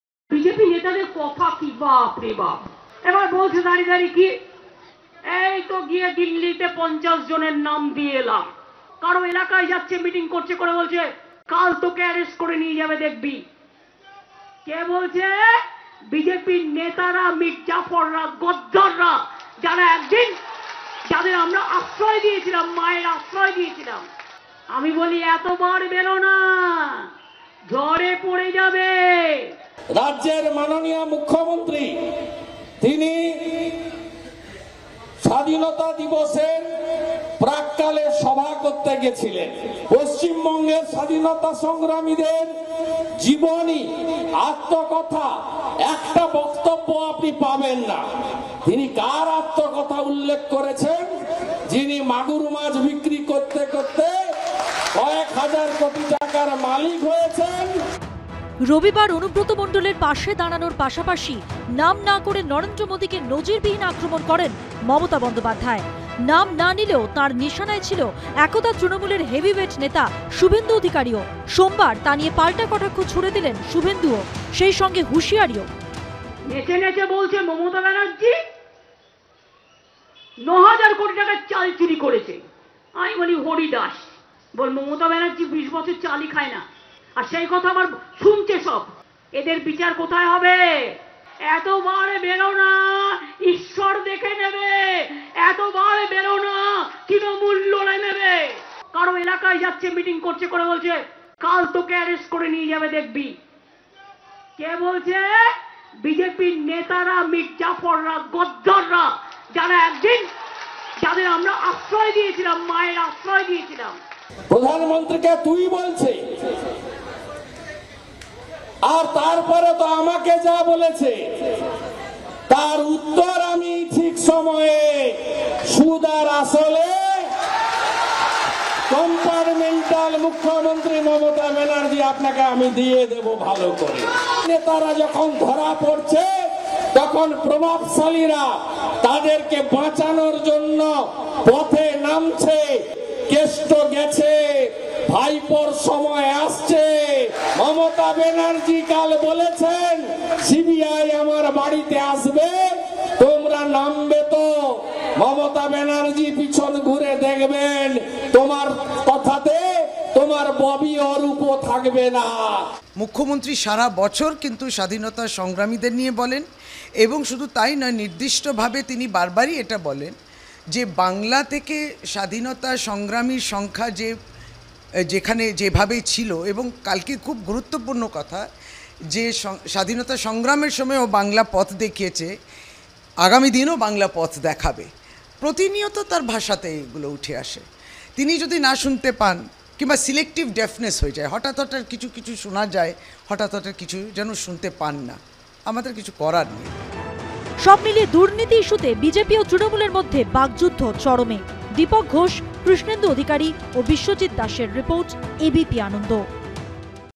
Suvendu Counters Mamata : মমতাকে পাল্টা শুভেন্দুর, আরও হেভিওয়েটের গ্রেফতারির হুঁশিয়ারি
নাম না করে আক্রমণ শানিয়েছিলেন মমতা বন্দ্যোপাধ্যায়। এবার তার পাল্টা জবাব দিলেন শুভেন্দু অধিকারী। হুঁশিয়ারি দিলেন আরও হেভিওয়েটের গ্রেফতারির। যা নিয়ে শুভেন্দু অধিকারীকে কটাক্ষ করতে দেরি করেনি তৃণমূলও।